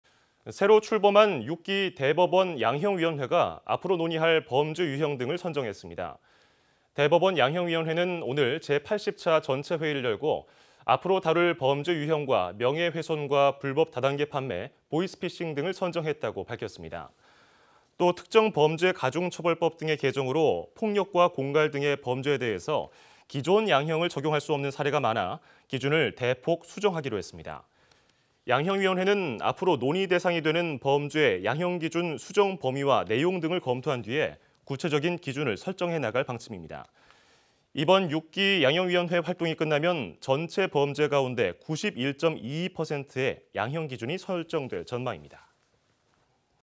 대법 양형위, 보이스피싱 등 양형기준 논의 / YTN 사이언스
새로 출범한 6기 대법원 양형위원회가 앞으로 논의할 범죄 유형 등을 선정했습니다.

대법원 양형위원회는 오늘 제80차 전체회의를 열고, 앞으로 다룰 범죄 유형에 명예훼손과 불법 다단계판매, 보이스피싱 등을 선정했다고 밝혔습니다.

또 특정범죄가중처벌법 등의 개정으로 폭력과 공갈 등의 범죄에 대해 기존 양형을 적용할 수 없는 사례가 많아, 기준을 대폭 수정하기로 했습니다.

양형위원회는 앞으로 논의 대상이 되는 범죄의 양형기준 수정 범위와 내용 등을 검토한 뒤 구체적인 기준을 설정해 나갈 방침입니다.

이번 6기 양형위의 활동이 끝나면 전체 범죄 가운데 91.22%에 양형기준이 설정될 전망입니다.